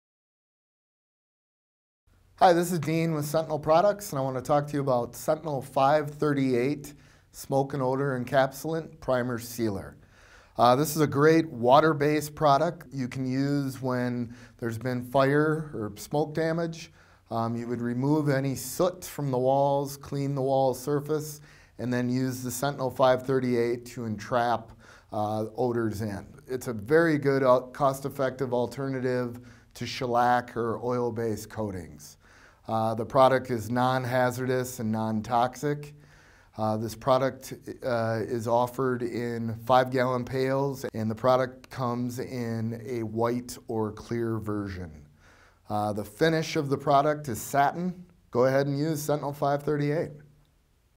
Sentinel Formula 538 Smoke & Odor Encapsulant - Jon-Don Video
Sentinel Formula 538 Smoke & Odor Encapsulant Primer/Sealer is a water-based product ideal for use after severe smoke and fire damage. After cleaning surfaces and removing as much soot and smoke residue as possible, Formula 538 can be applied to trap odors in, locking them away for good.

Formula 538 is a great alternative to shellac or oil-based coatings. It's non-hazardous and non-toxic. It can be applied with an airless sprayer or brush, and leaves a satin finish.

For more information on the Formula 538 and other high-quality concrete surface prep products at Jon-Don, visit us or call .